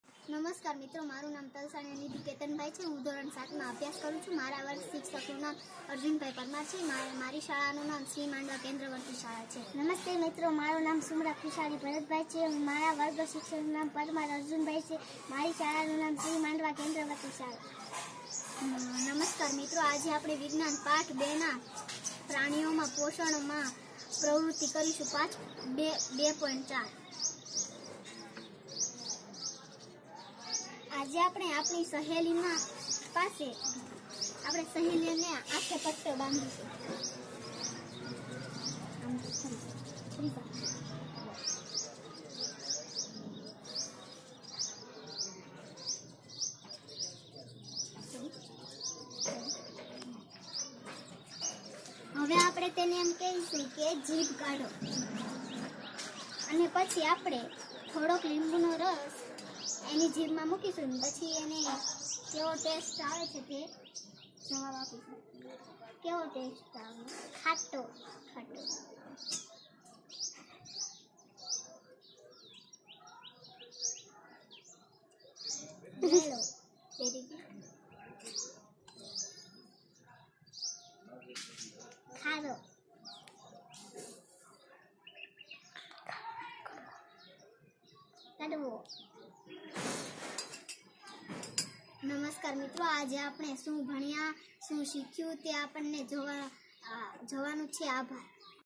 Ch.2 (2.4) Check the region of the different taste on tongue.(જીભ પરના ભાગો પર સ્વાદની ચકાસણી)
*Mandava K.V School*
👉Ta          :- Gadhda
👉Village :- Mandava
👉Dist.      :- Botad
👉Teacher:- Parmar Arjun A.
👉Student :-Talsaniya Nidhi  and Sumra Khushali
👉STD:-  7
👉Activity :-Ch.2 (2.4) Check the region of the different taste on tongue.(જીભ પરના જુદા જુદા ભાગો પર સ્વાદની ચકાસણી)

👉There is no limit to learn.Student clear their learning outcomes without any science equipment.Activity at home under guidance of teacher Arjun Parmar village Mandava.Education is most powerful weapon which you can use to change the world.